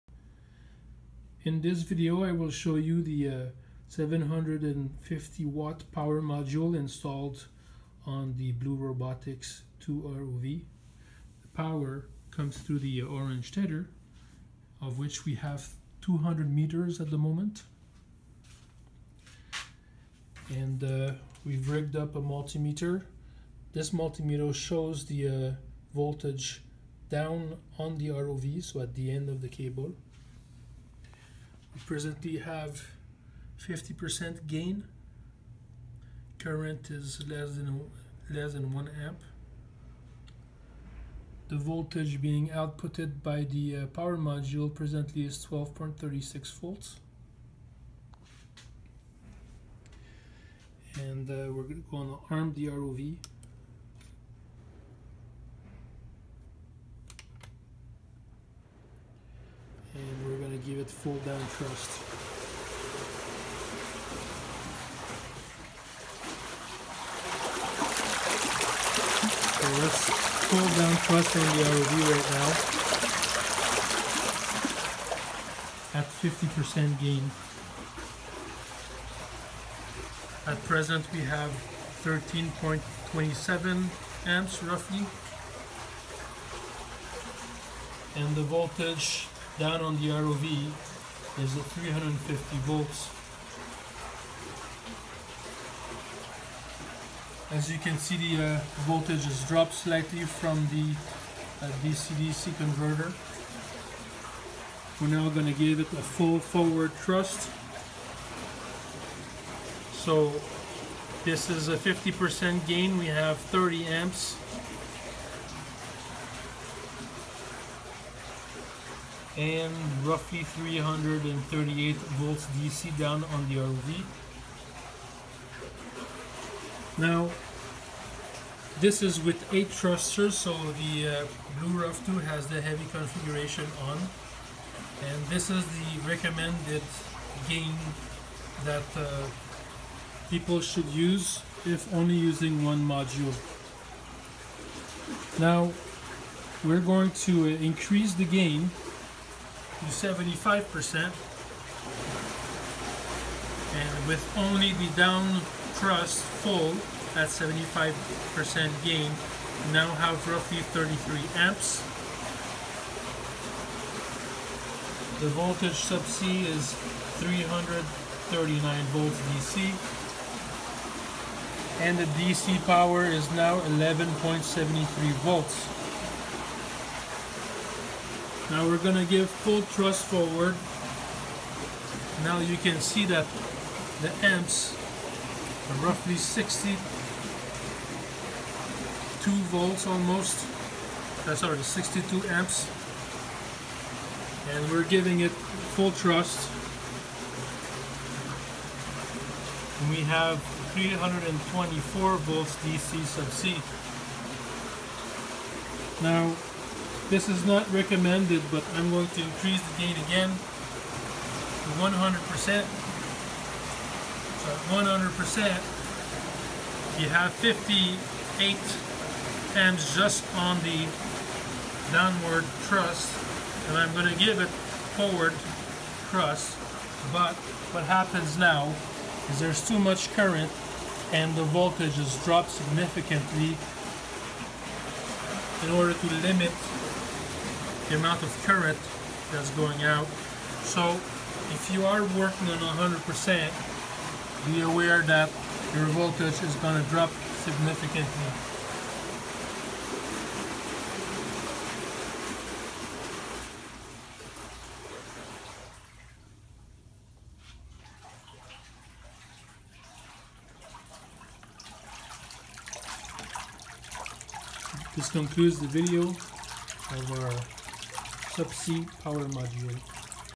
BlueROV2 Subsea Power Module 750W
Video showing the Delta ROV 750W Subsea Power Module.

Surface power system is plugged in 230VAC outlet and outputting 365VDC.

BlueROV2 Heavy configuration system is used on 200m tether.